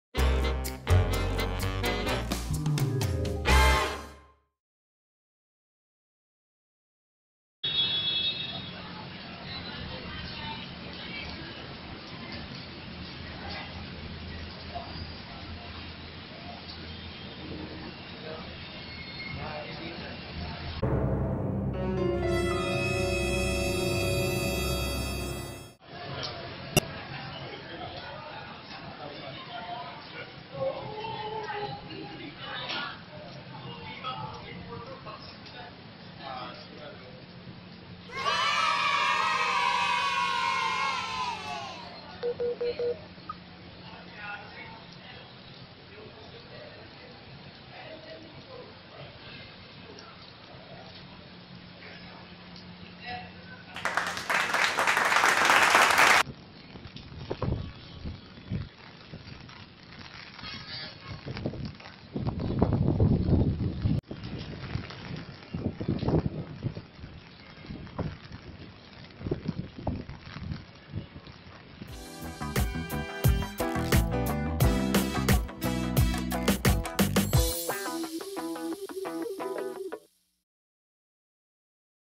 タリムの影響で交通機関マヒ in フィリピン 撮影中面白い職業見つけちゃいましたｗ
強い勢力の台風18号（タリム※）はフィリピンの東を西北西に進んでいます。 明日13日(水)は沖縄の先島諸島に最接近する予想です。

先島諸島では暴風や高波に警戒が必要です。

15日(金)頃には中国大陸の華南～華東に上陸する可能性が高く、その後は東へ向きを変え、九州に接近する可能性も出てきました。随時、最新の情報をご確認ください。

※フィリピンの言葉で「鋭い刃先」